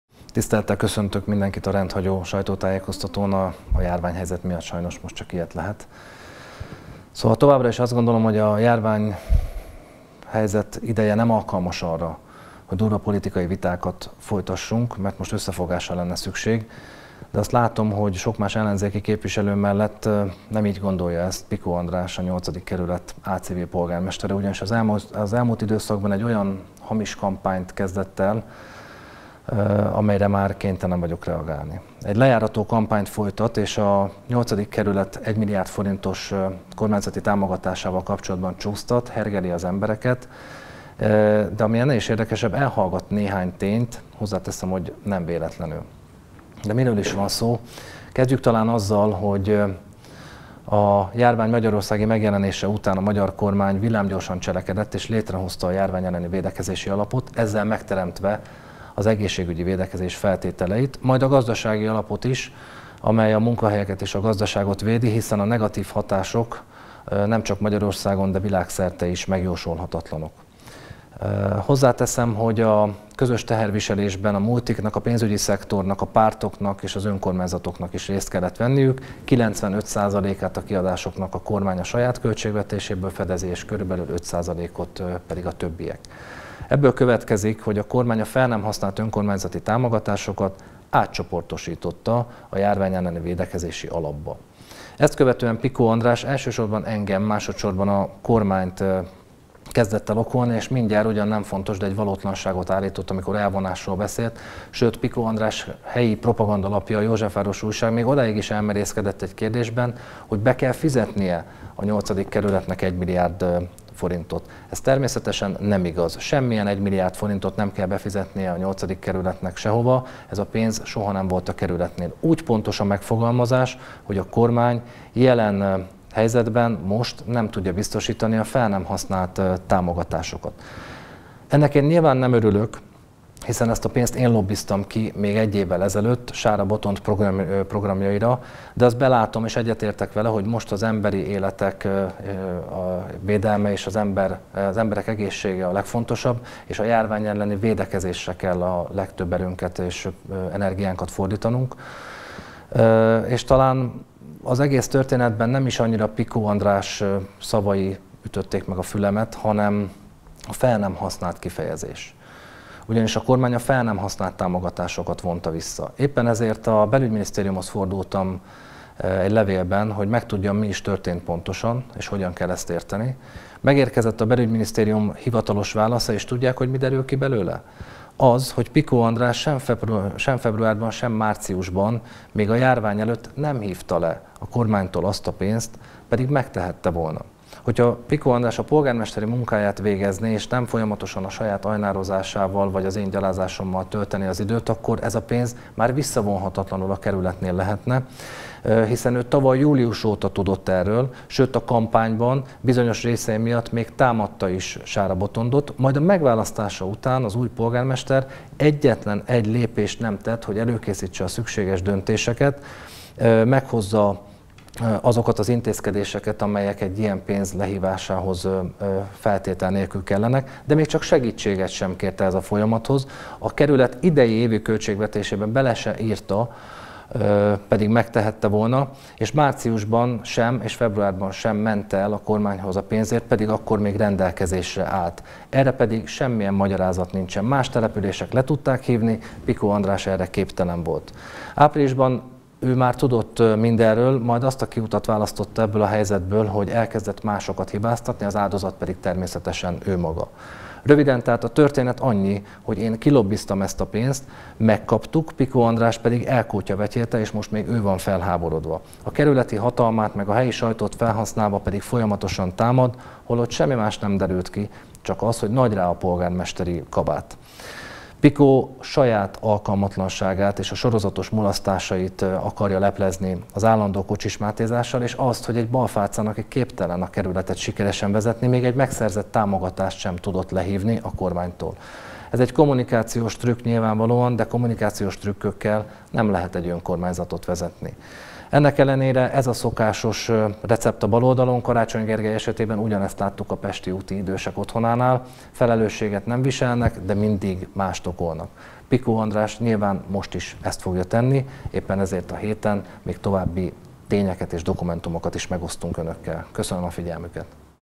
Kocsis Máté, a Fidesz frakcióvezetőjének online sajtótájékoztatója
Elkótyavetyélte Józsefváros támogatását Pikó András, a kerület álcivil polgármestere - hangsúlyozta Kocsis Máté, a Fidesz parlamenti frakcióvezetője online sajtótájékoztatóján.